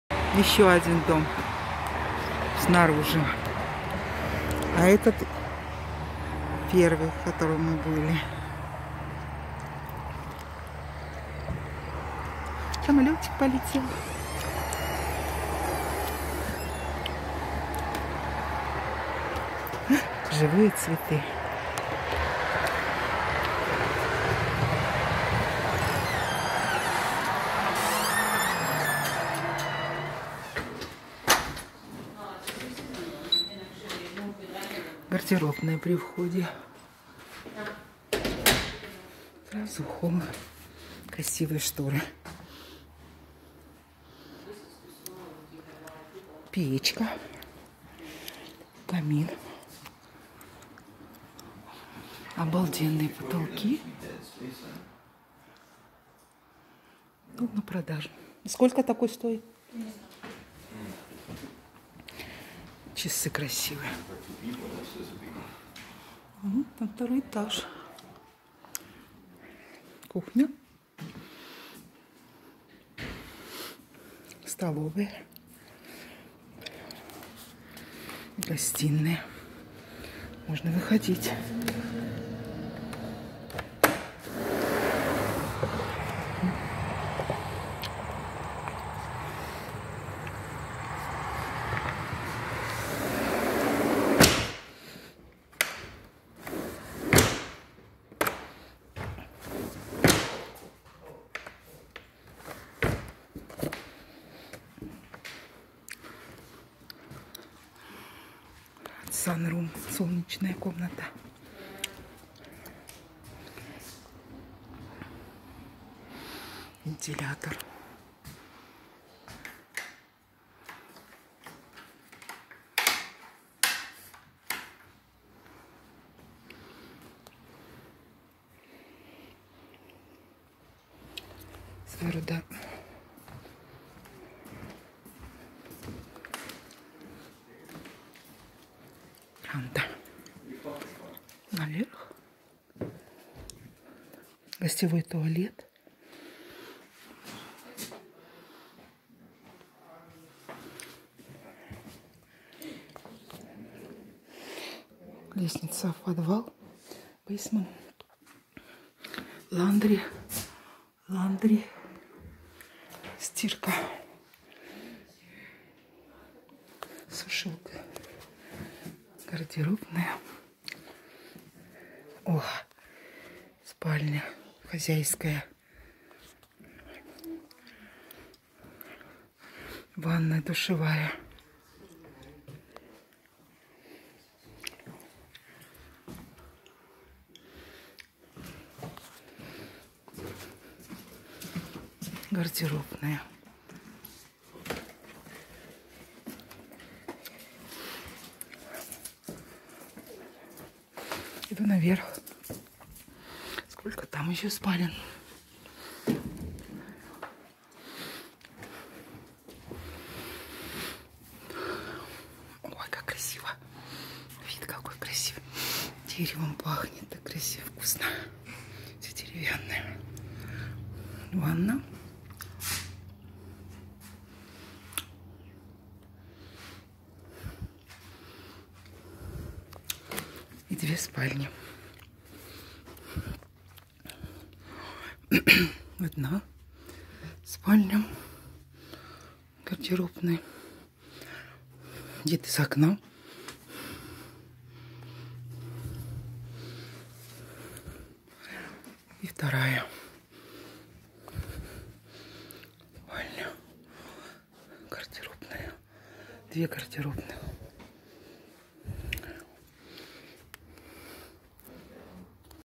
№53 Как купить дом...не дороже феррари.Откртый дом. Экскурсия в красивый дом
Домов на выставку пять и все разные  по планировке и дизайну и все замечательные напишите, какой больше понравился?